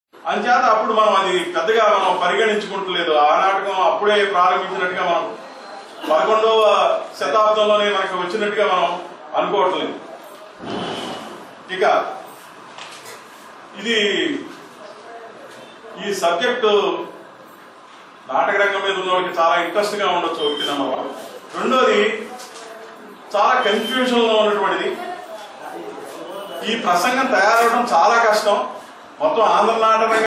KOTHAPALLI BANGARA RAJU SPEACH ON TELUGU DRAMITICAL LITERATURE
గ్రంధాలయ వారోత్సవాలలో భాగంగా  ఇండియన్ హైకూ క్లబ్ ఆధ్వర్యంలో 2012 నవంబర్ 18 వ  తేదీన  అనకాపల్లి  శాఖా గ్రంధాలయంలో శ్రీ కొత్తపల్లి బంగార రాజు ఉపన్యాసకులుగా " తెలుగు  సాహిత్యం - ప్రదర్శన' అనే అంశంపై సాహిత్య సమావేశం జరిగింది  ఈ  సమావేశానికి అధ్యక్షునిగా క్లబ్ ఉపాధ్యక్షులు శ్రీ గట్టి బ్రహ్మాజీ, ముఖ్య  అతిథిగా జిల్లా గ్రంధాలయ చైర్మన్ శ్రీ  తోట నగేష్, ఆత్మీయ అతిథిగా సిహెచ్.లక్ష్మి సాహితీ శిరీష గ్రంధాలయ అధికారిణి  పాల్గొన్నారు.